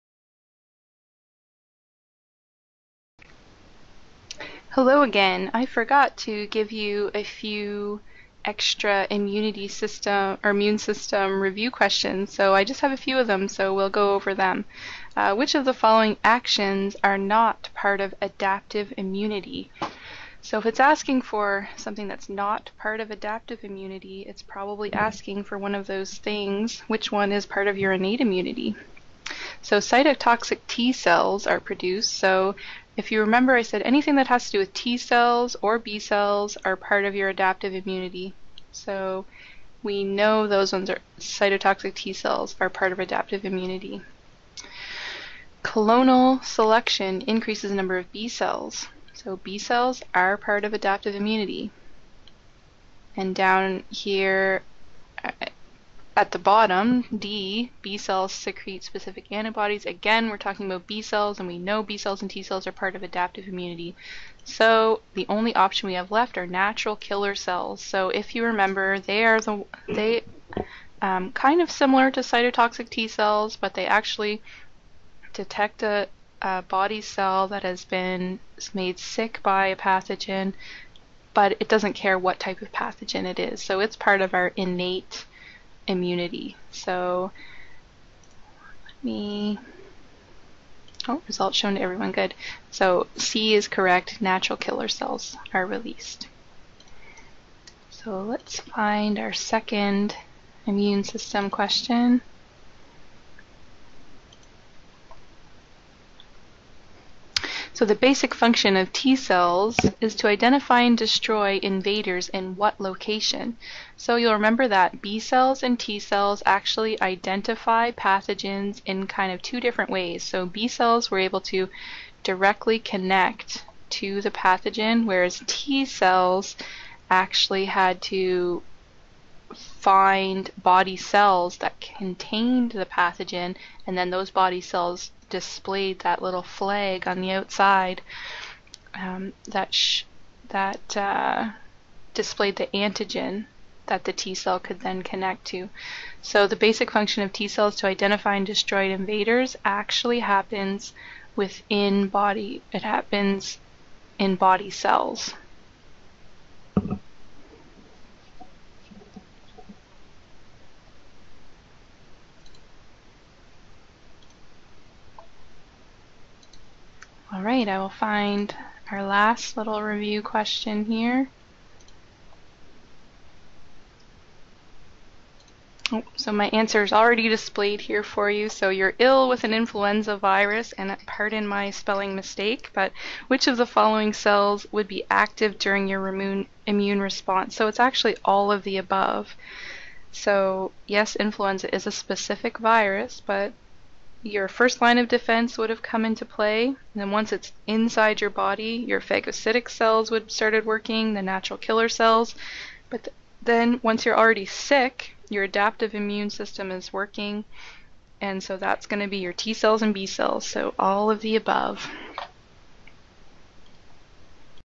Biology 1047 Immune system practice Tuesday, July 3, 2018 3 43 35 PM
A few review questions I forgot to include in the main recording.